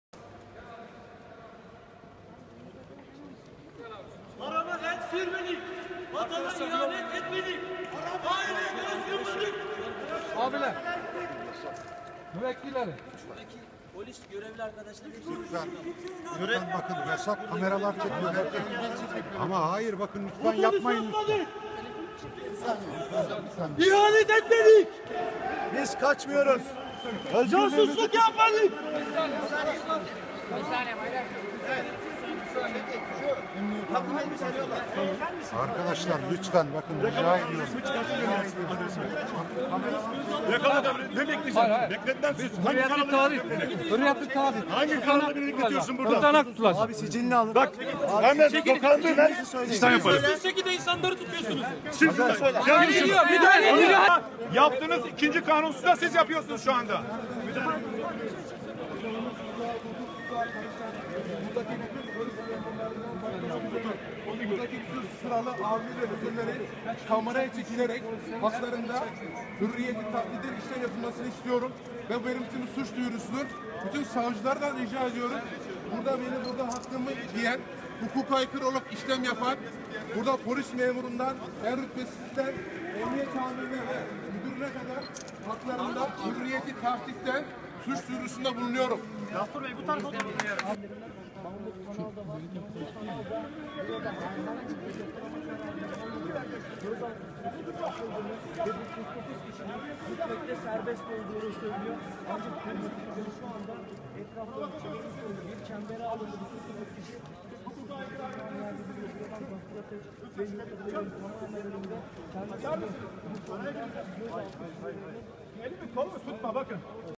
Polise Adliyede Abluka!
Yolsuzluk ve terörle mücadele eden polisleri hedef alan sahur operasyonu kapsamında mahkemeye sevk edilen 49 polis gözaltı süreleri dolmasına rağmen adliyede tutulmaya devam ediliyor. Aralarında eski terör şube müdürü Yurt Atayün ve Ömer Köse’nin de olduğu 49 polis avukatlarıyla birlikte adliyeden ayrılmak üzere mahkemenin bulunduğu kattan ayrıldı.